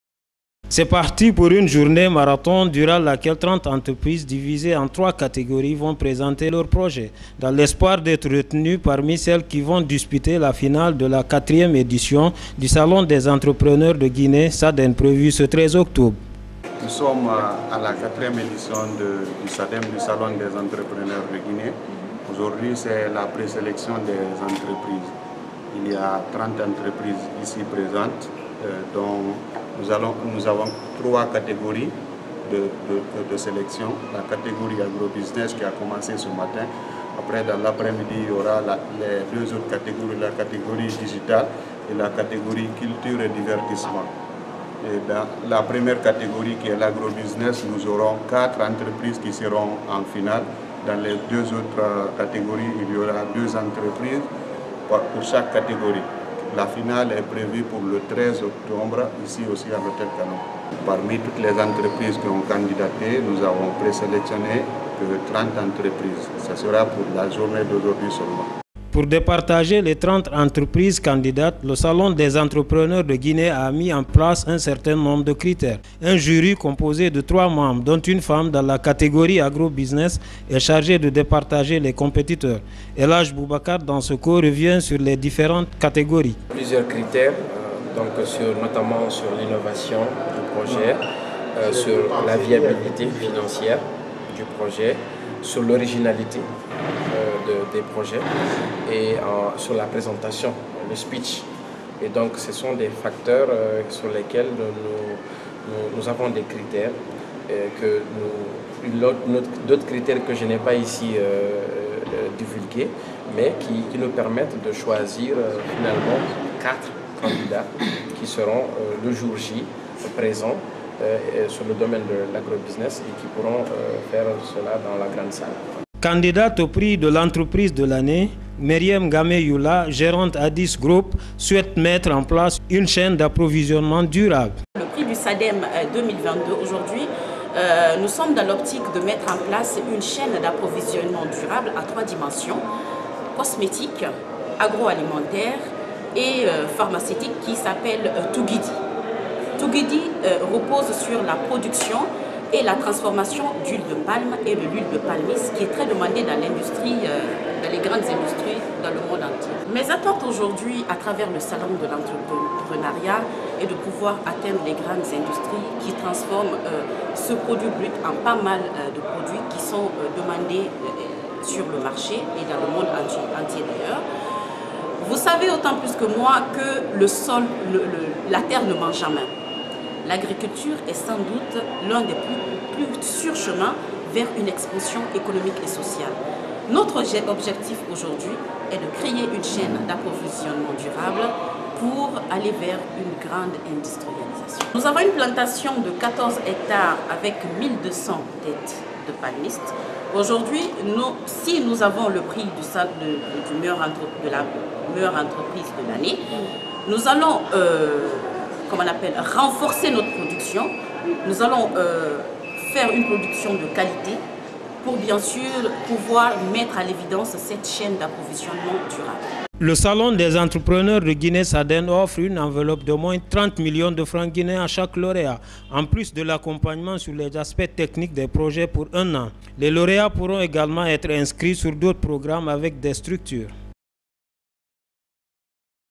La présélection pour la 4ème édition du Salon des Entrepreneurs de Guinée (SADEN) lancée
La présélection au compte de la quatrième (4ème) édition du Salon des entrepreneurs de Guinée (SADEN) se tient ce samedi, 13 août 2022, dans un réceptif hôtelier de Conakry. Trente (30) entreprises en compétition présentent leurs projets à un jury chargé de sélectionner, selon des critères établis, les huit (8) meilleures qui vont s'affronter en finale. La cérémonie se tiendra pour toute la journée de ce samedi, a appris sur place Guineematin.com à travers un de ses reporters.

C'est parti pour une journée marathon durant laquelle trente (30) entreprises, divisées en trois (3) catégories, vont présenter leurs projets dans l'espoir d'être parmi celles qui vont disputer la finale de la quatrième édition du salon des entrepreneurs de Guinée (SADEN) prévue le 13 octobre à Conakry. Le thème retenu cette année est « l'entreprenariat féminin, moteur de développement socio-économique et de la croissance inclusive ».

Boubacar Diallo, membre de l’équipe SADEN

Boubacar Diallo, membre de l’équipe SADEN, a apporté des détails sur ce qui est prévu au cours de cette journée. « Aujourd'hui, c'est la présélection des entreprises. Il y a 30 entreprises présentes. Nous avons 3 catégories de sélection : la catégorie Agrobusiness qui a commencé ce matin ; dans l'après-midi, il y aura la catégorie Digitale, et la catégorie Culture et Divertissement. Dans la première catégorie, Agrobusiness, nous aurons quatre (4) entreprises qui seront en finale. Dans les autres catégories, il y aura deux entreprises pour chaque catégorie. La période de la présélection va durer toute la journée d'aujourd'hui et parmi toutes les entreprises qui ont soumissionné, nous avons présélectionné 30 », a-t-il expliqué Boubacar Diallo, tout en précisant que l'annonce des huit (8) finalistes sera faite le 17 août prochain.

Pour départager les 30 entreprises candidates, le salon des entrepreneurs de Guinée a mis en place un certain nombre de critères. Elhadj Boubacar Dansoko, un des trois (3) membre du jury dans la catégorie Agrobusiness, est revenu sur les critères qui vont départager les concurrents.

Elhadj Boubacar Dansoko, membre du jury

« Nous avons plusieurs critères, notamment sur l'innovation du projet, sur la viabilité financière du projet, sur l'originalité des projets et sur la présentation, le speech, etc. Donc, ce sont des critères qui nous permettent de choisir finalement quatre (4) candidats qui seront le jour-j présents sur le domaine de l'Agrobusiness et qui pourront faire cela dans la grande salle », a-t-il indiqué.

Candidate au prix de l'entreprise de l'année, Mariam N'gamet Youla, gérante de ADASY GROUP, souhaite mettre en place une chaîne d'approvisionnement durable. Et si elle remporte le prix, elle ambitionne d'atteindre les grandes industries pour pouvoir vendre ses produits à travers le monde.

Mariam N'gamet Youla, gérante de ADASY GROUP

« Pour le prix du SADEN 2022, nous sommes dans l'optique de mettre en place une chaîne d'approvisionnement durable à trois (3) dimensions : cosmétique, agro-alimentaire, et pharmaceutique, qui s'appelle Touguidi. Touguidi repose sur la production et la transformation d'huile de palme et de l'huile de palmiste qui est d'ailleurs très demandée dans les grandes industries du monde entier. Mes attentes, à travers le salon de l'entreprenariat, c’est de pouvoir atteindre les grandes industries qui transforment ce produit brut en pas mal de produits qui sont demandés sur le marché dans le monde entier. Vous savez autant que moi que la terre ne ment jamais et que l'agriculture est sans doute l'un des plus sûrs chemins vers une expansion économique et sociale. Notre seul objectif aujourd’hui est de créer une chaîne d'approvisionnement durable pour aller vers une grande industrialisation. Nous avons une plantation de 14 hectares avec 1200 têtes de palmistes et si nous obtenons le prix de la meilleure entreprise, nous allons renforcer notre production, faire une production de qualité pour bien-sûr pouvoir mettre à l'évidence cette chaîne d'approvisionnement durable », a-t-elle fait savoir.

À noter qu’en plus de l'accompagnement sur les aspects techniques des projets pour un an, le Salon des entrepreneurs de Guinée (SADEN) offre une enveloppe de 30 millions GNF à chaque lauréat. Ils pourront également être inscrits sur d'autres programmes avec des structures évoluant dans le même domaine.